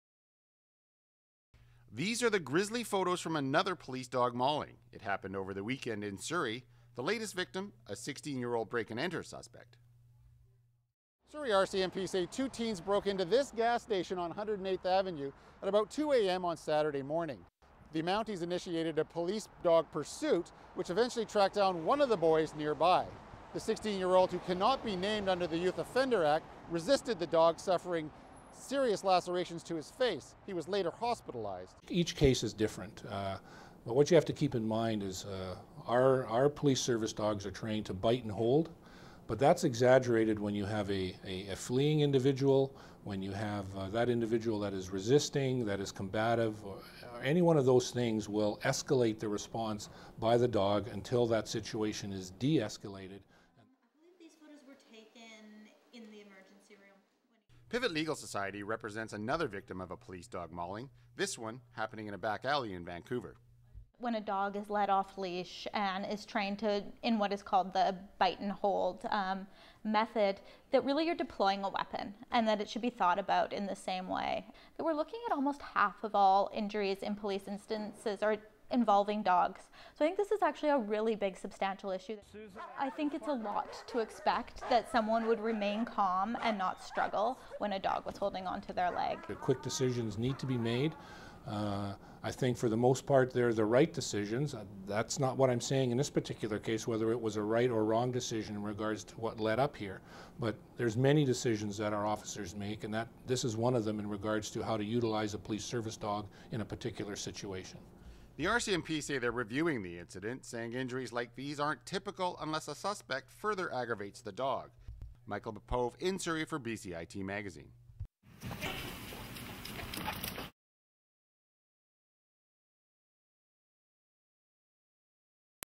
There's Been Another Police Dog Mauling, This Time in Surrey
There's been another police dog mauling in the Lower Mainland. This time in Surrey on a 16 year old break and enter suspect. Michael Popove has the story. A warning, some of the photos in this story, may be disturbing.